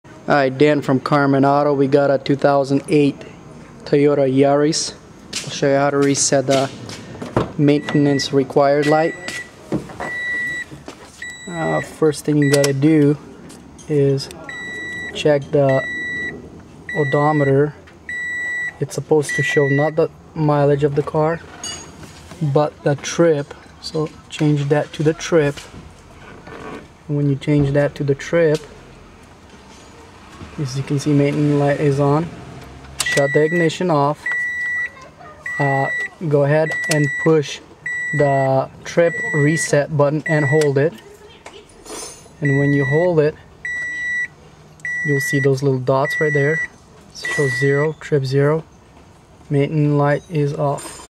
How To Reset Toyota Yaris Oil Light - Yaris Service Reset - Maint Req'd Reset
In this How To Reset Toyota Yaris Oil Light - Yaris Service Reset - Maint Reqd Reset video I will show you how to reset your maint reqd light simple no tools needed this will reset your oil interval every time you do oil change do this to reset light and next time your oil needs a change it will remind you